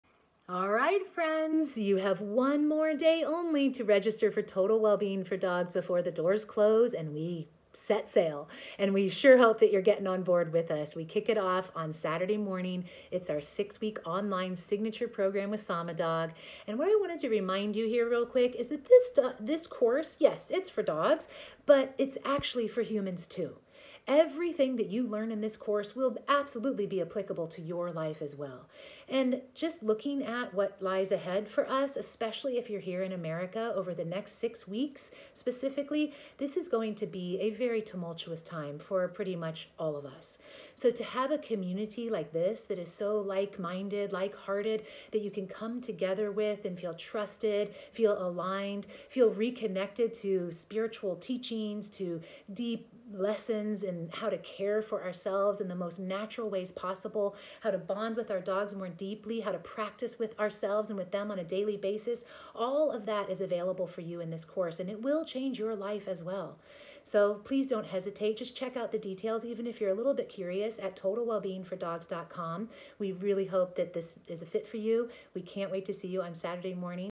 Doors Closing for Total Wellbeing for Dogs Course
2020 Enrollment closes on Thursday, October 8, 2020 @ midnight PT

Learn the 6 Pillars of a Truly Balanced Canine

General Rules: 1. No profanity |  2. Be respectful to/about humans and animals